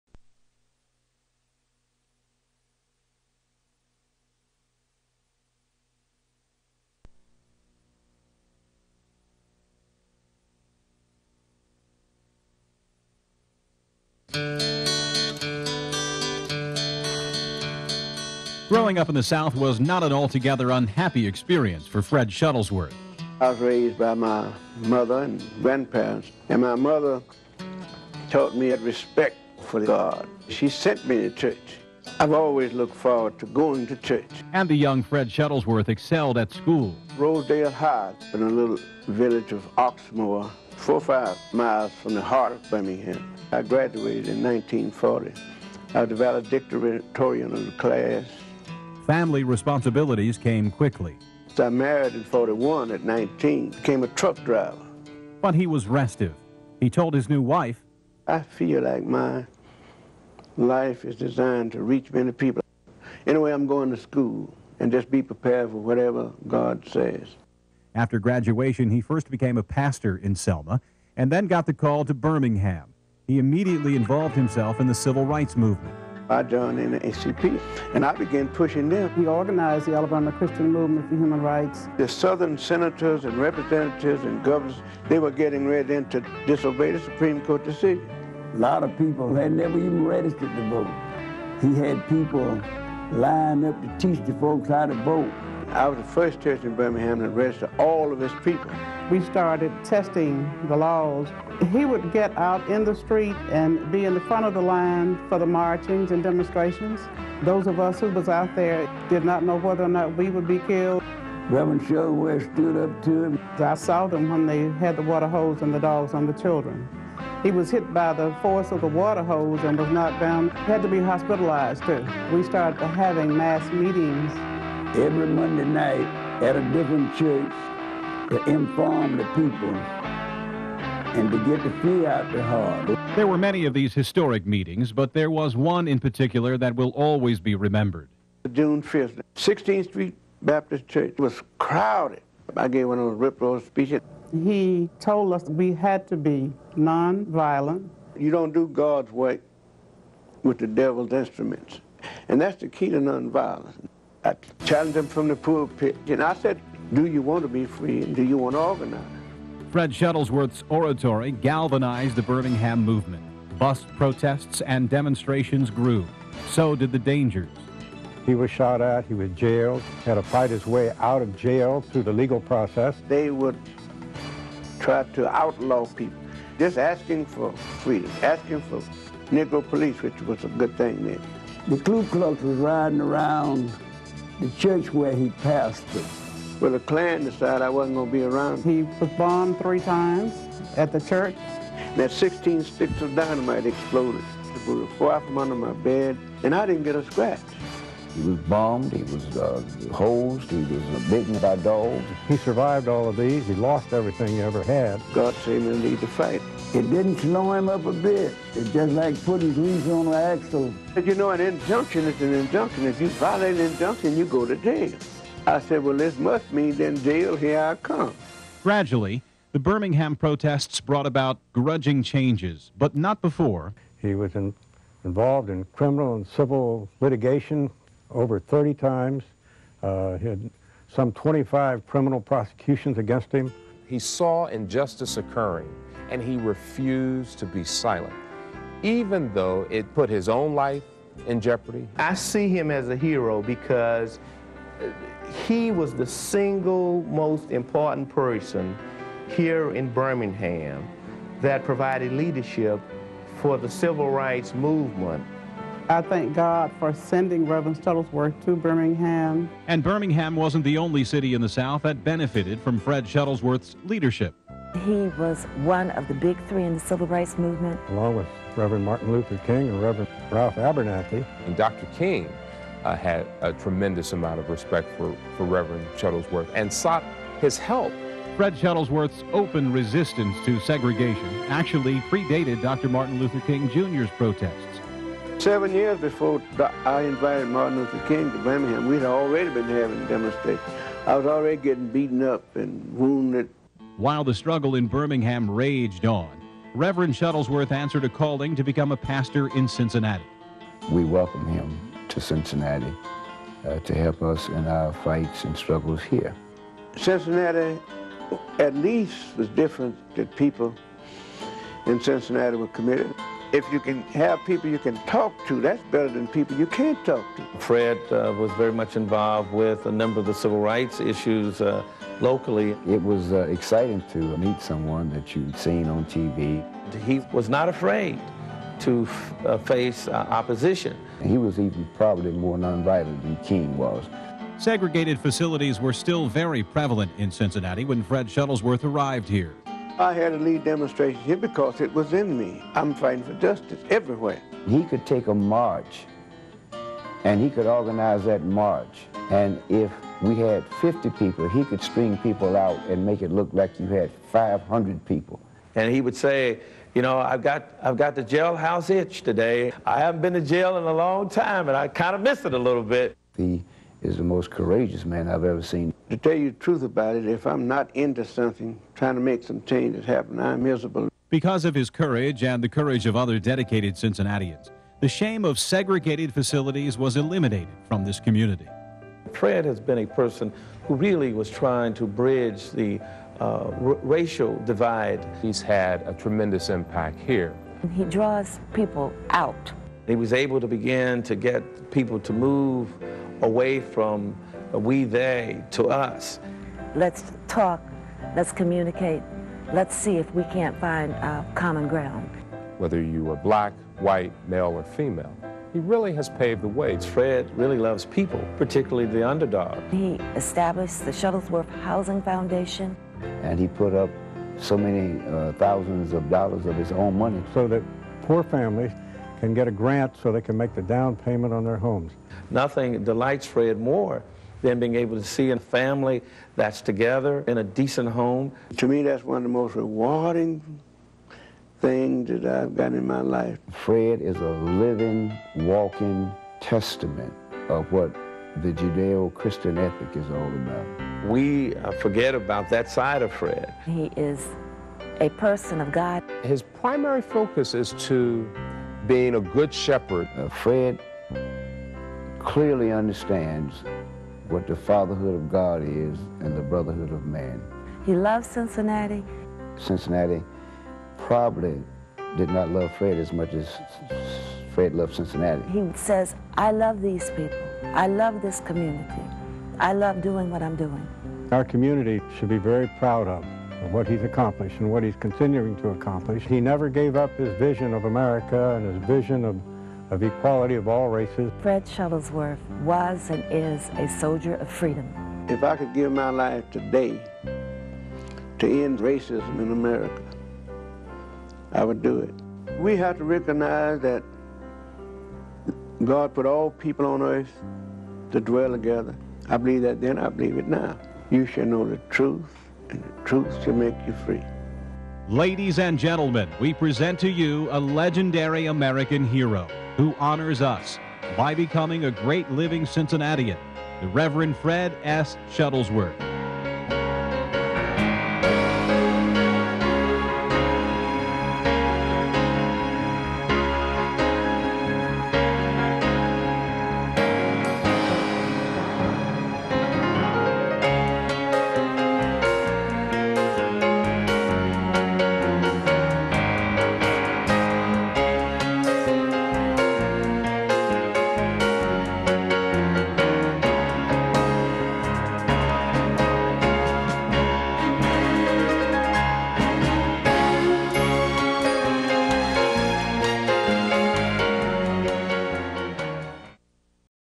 Rev. Fred Shuttlesworth, 2000 Great Living Cincinnatian
Rev. Shuttlesworth was a Civil rights activist during the 1950s and 1960s, and pastor of Greater New Light Baptist Church in Cincinnati from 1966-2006.